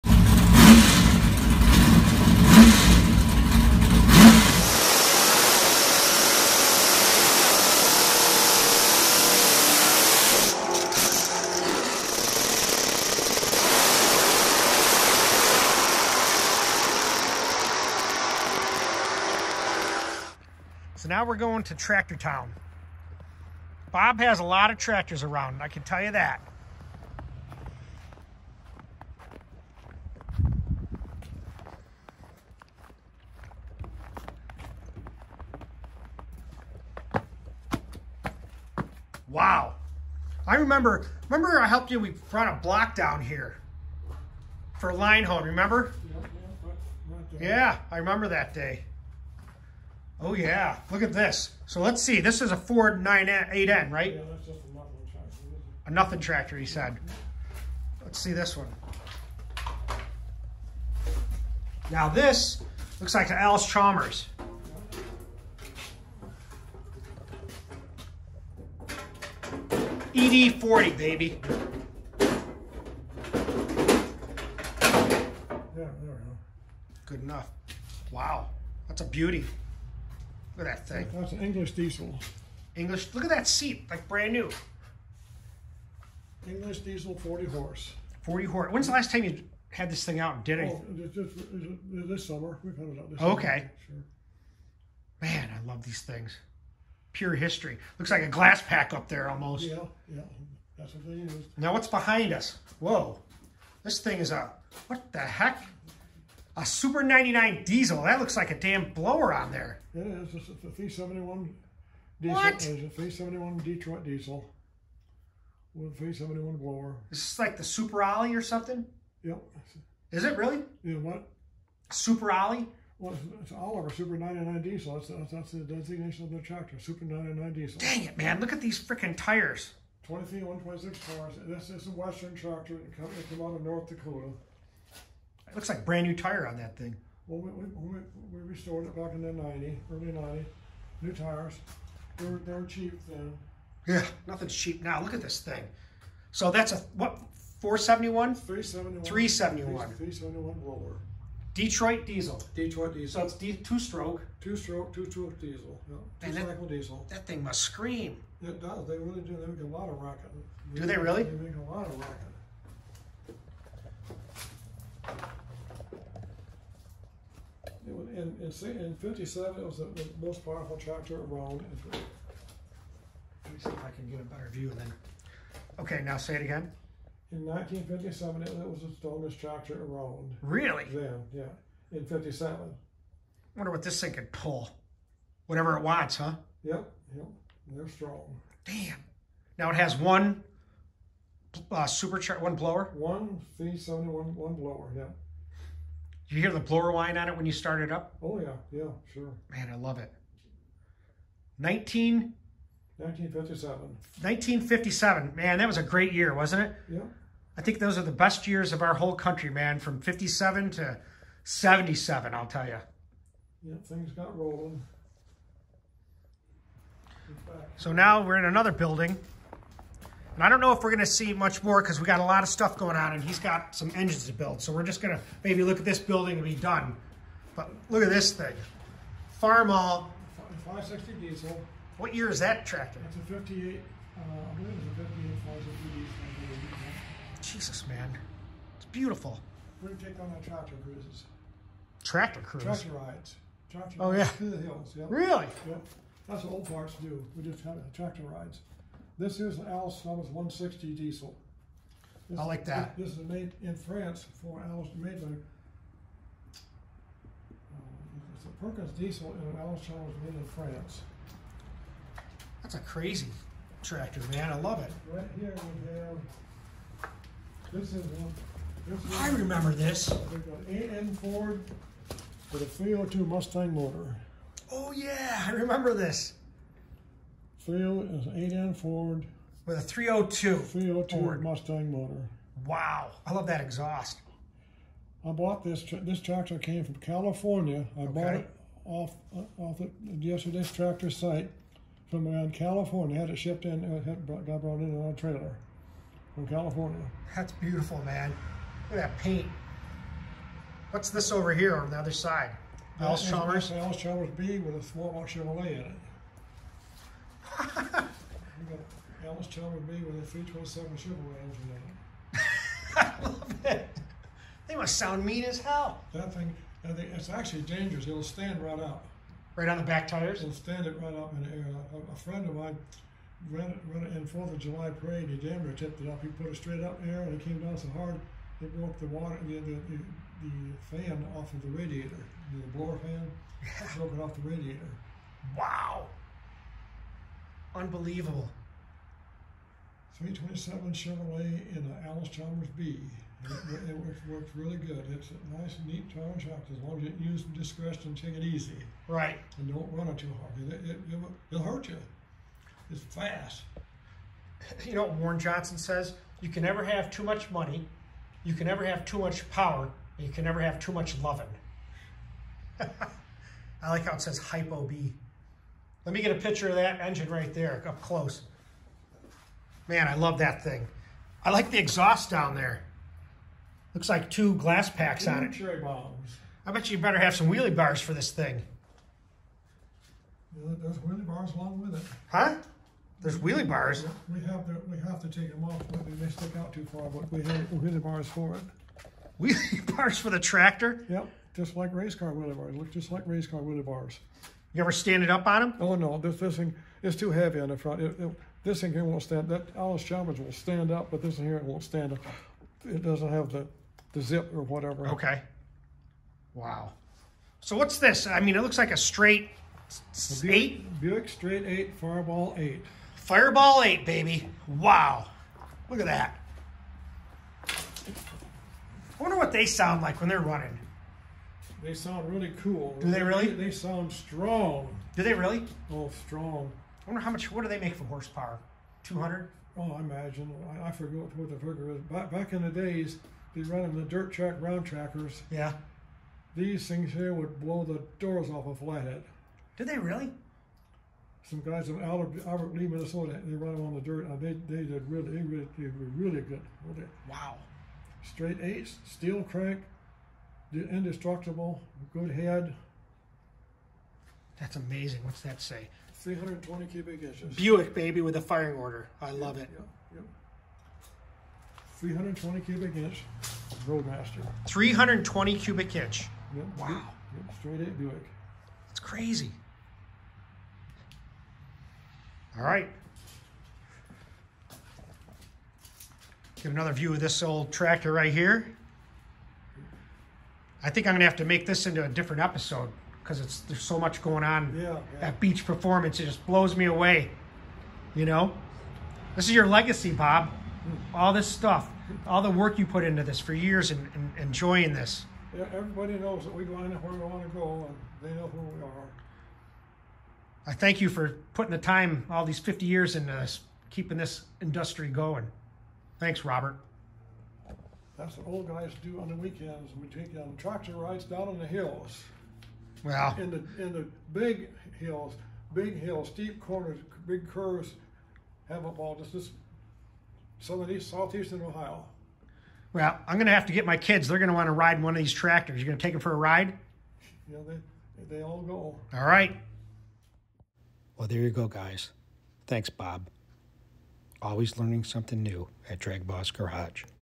DBG: Tractor Town Bob's Ford 302 8N, Chevy 327 Allis- Chalmers B Blown 57 Oliver, Fire Ball Buick 8
Come along to Tractor Town, More stories with Bob. This video shows a quick view of some of his tractor collection. It is a quick tour and you will see his GMC Blown 1957 Oliver Super 99, Big Ollie don't play, she is a worker. This tractor has from the factory a a 371 GMC blower, these were installed on the 2 stroke Detroit Diesels, since there are no intake valves. This is were the blowers we all love to see and hear on Hot Rods. They all came from Tractors. Also is an English 40 HP Diesel in a Allis-Chalmers ED-40, a real beauty. There is also a work in progress of what he calls a nothin tractor, an old Ford 8N.
Next up is a 1969 Allis-Chalmers 160, these are all in mint condition, followed by a Flamed Ford 8N, with a Ford Mustang 302, and a Mean Machine Allis-Chalmers HiPo B, with a Chevy 327. These tractors are Bad. Bob said the HiPo B will yank the front tires off the ground to the point of rolling over, they both have wheelie bars. He tells the story about the wheel standing Allis- Chalmers. Also seen is a Farmall 560.
The last surprise is a 320 ci FireBall Buick straight 8. Bob syas these are Stronggg engines, he said back in the dirt track days, they used put a spanking on the Ford Flatheads, strong blocks, steel crank, ain't no Joke That FireBall straight 8.
Always learning something new at DragBoss Garage